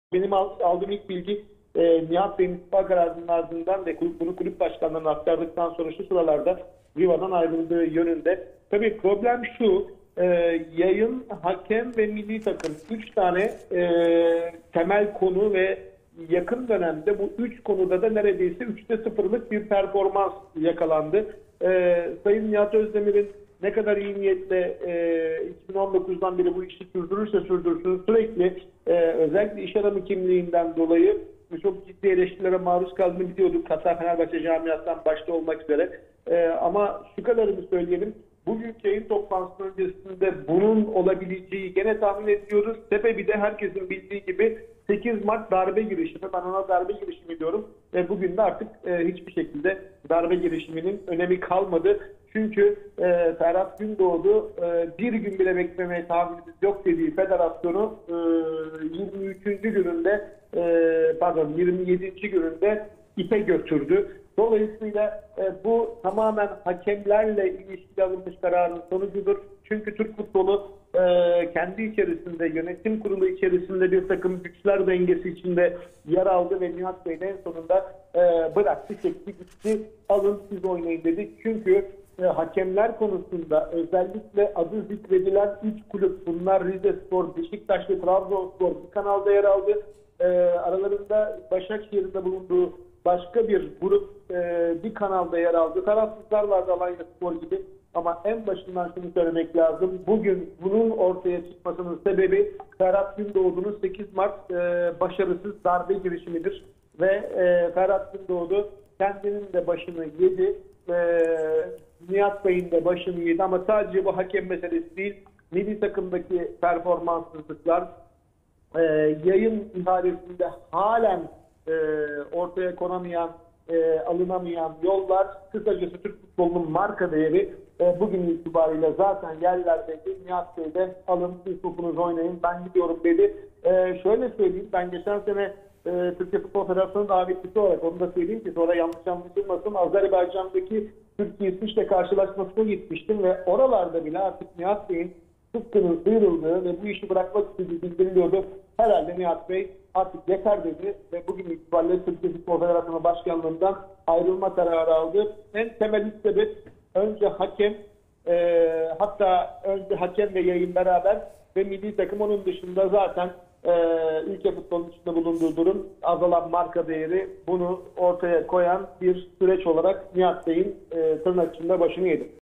Nihat Özdemir'in istifa kararını Mehmet Ayan yorumladı!
TFF Başkanı Nihat Özdemir'in istifa kararını Mehmet Ayan değerlendirdi: "Yayın, hakem ve milli takım. Son dönemde 3 konuda da 3'te 0'lık performans yakalandı."